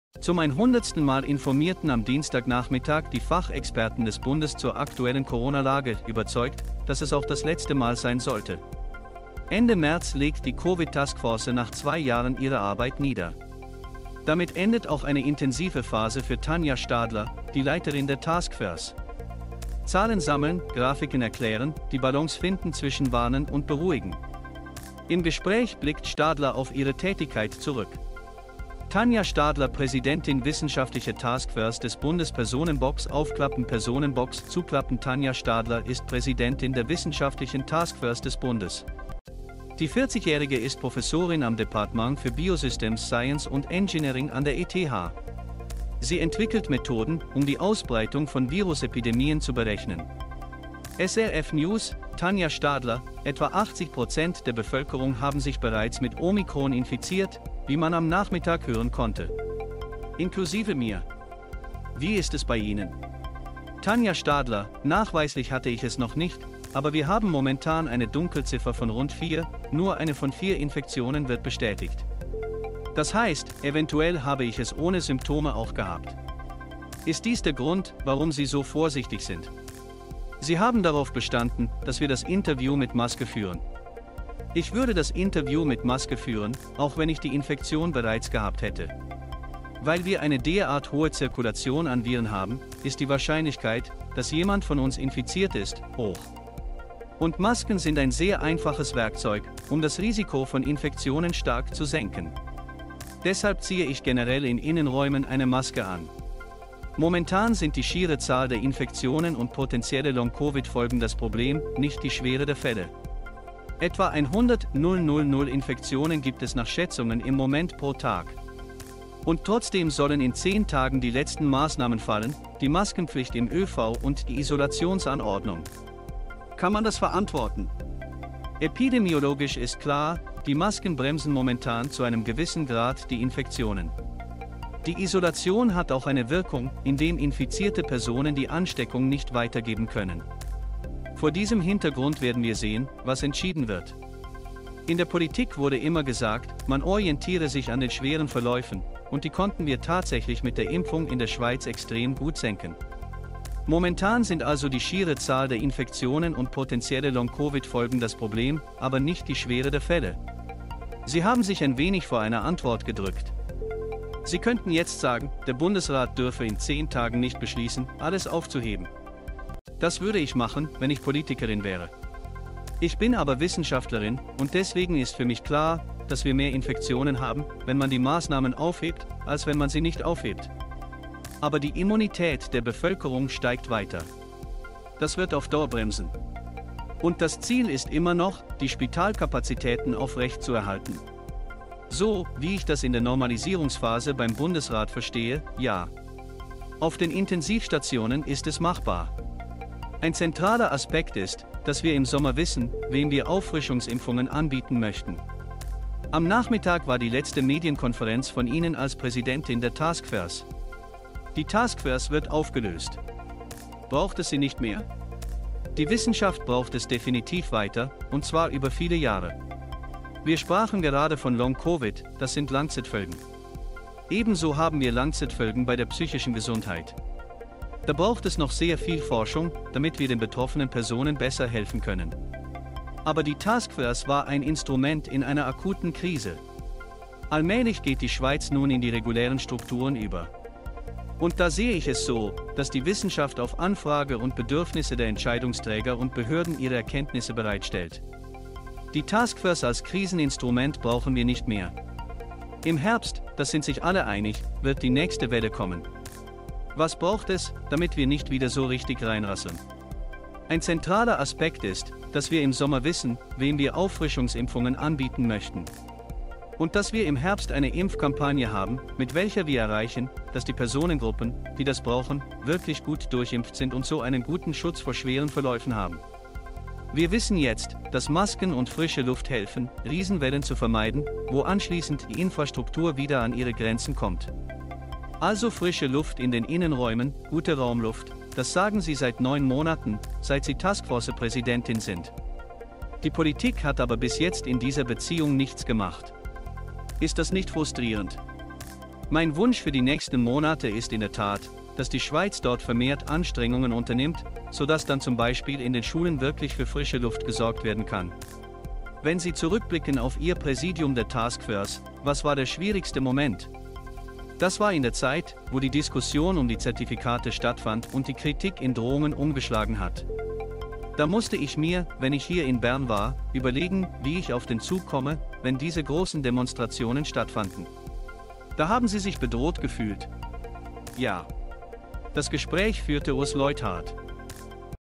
Letzter Auftritt des Arbeitskreises - Tanja Stadler: „Aus Kritik wird Drohung“
Nach zwei Jahren ist die Covid-Taskforce der Bundesregierung zu Ende. Präsidentin Tanja Stadler führt eine erste Bestandsaufnahme durch.
Am Dienstagnachmittag informierten Experten des Bundes zum 100. Mal über die aktuelle Corona-Situation – und glauben, dass es das letzte Mal sein dürfte. Ende März geht die Covid-Task Force nach zwei Jahren in den Ruhestand. Damit ging auch für Arbeitsgruppenleiterin Tanja Stadler eine angespannte Phase zu Ende. Sammeln Sie Zahlen, interpretieren Sie Grafiken und finden Sie ein Gleichgewicht zwischen Warnung und Komfort. Im Interview ließ Stadler ihre Arbeit Revue passieren.

Tanja Stadler Vorsitzende des Arbeitskreises Wissenschaft des Bundes Öffnen Persönliche E-Mail Schließen Persönliche E-Mail Tanja Stadler ist Vorsitzende des Arbeitskreises Wissenschaft des Bundes. Der 40-Jährige ist Professor am Departement Biosystems Science and Engineering der ETH. Sie entwickelte Methoden zur Berechnung der Ausbreitung einer Virusepidemie.

SRF News: Tanja Stadler hat, wie am Nachmittag zu hören war, rund 80% der Bevölkerung mit omicron infiziert. mich eingeschlossen. Wie geht es dir?

Tanja Stadler: Es stellt sich heraus, dass ich die Infektion nicht hatte, aber wir haben derzeit etwa vier Dunkelziffern und nur ein Viertel der Infektionen sind bestätigt. Das bedeutet, dass ich möglicherweise keine Symptome habe.

Bist du deshalb so vorsichtig? Sie bestanden darauf, dass wir Maske interviewen.

Auch wenn ich infiziert bin, werde ich ein Interview mit Maske führen. Da unsere Virusübertragungsrate so hoch ist, besteht eine hohe Wahrscheinlichkeit, dass einer von uns infiziert wird. Und Masken sind ein sehr einfaches Hilfsmittel, das das Infektionsrisiko stark reduzieren kann. Deshalb trage ich in Innenräumen meistens eine Maske.

Im Moment ist die Frage die Zahl der Infektionen und mögliche langfristige Covid-Folgen, nicht die Schwere der Fälle.

Schätzungen zufolge infizieren sich derzeit täglich rund 100.000 Menschen. Die endgültigen Maßnahmen sollen jedoch innerhalb von zehn Tagen erfolgen, Maskenpflicht für den öffentlichen Verkehr und Quarantäneanordnungen. Können Sie das verantworten?

Eines ist epidemiologisch klar: Masken bremsen das Infektionsgeschehen derzeit etwas aus. Die Quarantäne verhindert auch, dass eine infizierte Person eine ansteckende Krankheit verbreitet. In diesem Zusammenhang werden wir sehen, was entschieden wird. In der Politik ist immer die Rede davon, sich an ernsthafte Kurse zu gewöhnen, und die konnten wir mit Impfungen in der Schweiz tatsächlich sehr gut reduzieren. Derzeit ist die absolute Zahl der inf